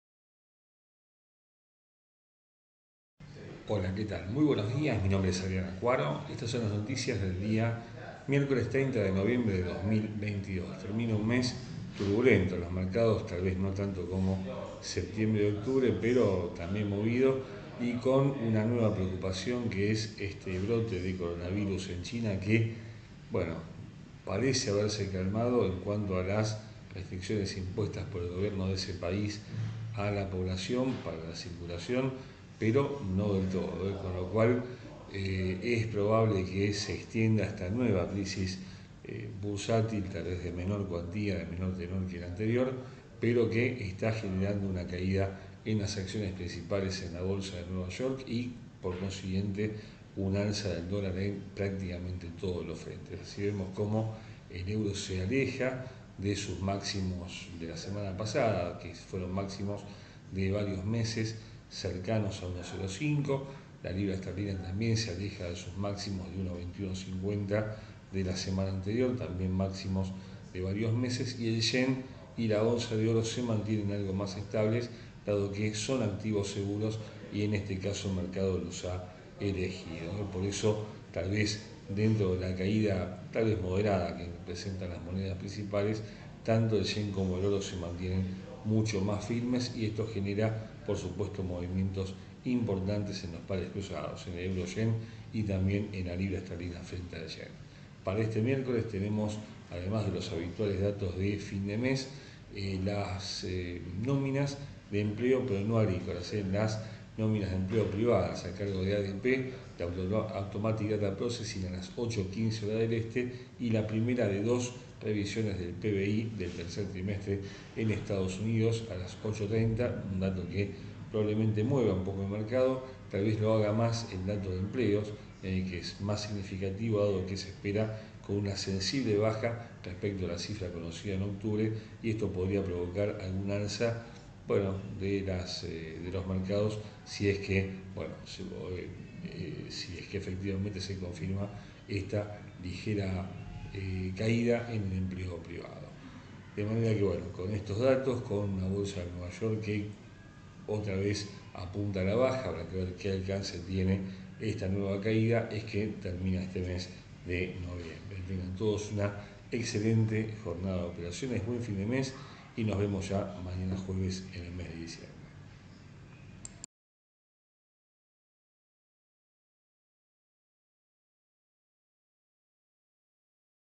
Las Noticias del Día Miércoles 30 de noviembre de 2022
El dólar domina, ante señales confusas desde China

Hola Amigos, buen día. Las protestas que tuvieron lugar en China durante el fin de semana contra la política del gobierno de ese país en materia sanitaria, dieron paso a una serie de anuncios no del todo claros. La población se queja de las restricciones que impone el gobierno del gigante asiático para tratar de bajar los contagios de coronavirus, que resurgieron en las últimas semanas en el país.
Después de varios incidentes, que derivaron en una crisis social, el gobierno intentó el martes enviar algunas señales de distensión. Algunas agencias oficiales sugirieron además que no es conveniente mantener las restricciones por mucho tiempo, por las consecuencias ya conocidas en los peores momentos de la pandemia.
Los mercados europeos reaccionaron positivamente a las medidas de relajación anunciadas, pero estas no alcanzaron para cambiar el mal humor en Wall Street. Los operadores parecen palpitar dificultades en la cadena de suministros en el futuro si las restricciones se extienden en el tiempo. Ello, por supuesto, repercutirá de lleno en el índice de inflación en Estados Unidos, el mismo que la Fed intenta bajar a como de lugar.
Justamente, a falta de dos semanas para el anuncio de política monetaria del banco central, se torna imprevisible saber que decisión tomará. Probablemente deberá evaluar si el problema en China persiste con el paso de los días, y en tal caso extenderá su política monetaria agresiva en diciembre, algo que estaba puesto en duda, y hasta descartado, por varios analistas.
La bolsa de Nueva York volvió a tener un día negativo, pese a que el inicio de las operaciones fue auspicioso. La caída de las acciones principales (con un gap incluido en todos los índices que quedó entre el cierre del viernes y la apertura del lunes), repercutió naturalmente en un alza del dólar, que se fortalece ante las monedas principales.
Así, el euro apunta a la zona de 1.0300, luego de haber rozado 1.0500 el lunes, con la libra esterlina también apuntando a la baja, y lejos de la zona de 1.2150 que ofreció como máximo de varios meses el viernes pasado. El yen se mantiene algo más firme, en la zona de 138.50, producto de que los mercados están eligiendo a la moneda nipona como refugio seguro, al igual que a la onza de oro, que buscó 1760.00 dólares en las primeras horas del martes.
Este miércoles comienza la parte más importante de la semana en materia de informes. A los datos habituales de fin de mes se suma la revisión del PBI del tercer trimestre, a las 8:30 del este. Unos minutos antes la firma ADP dará a conocer los datos de empleo del sector privado, con una baja sensible respecto a las cifras de octubre, lo cual podría repercutir favorablemente en los mercados y en contra del dólar
La bolsa de Nueva York volverá a ser el foco de la atención de los operadores, dado que ante un escenario complejo como el actual ofrecerá su cuota de volatilidad.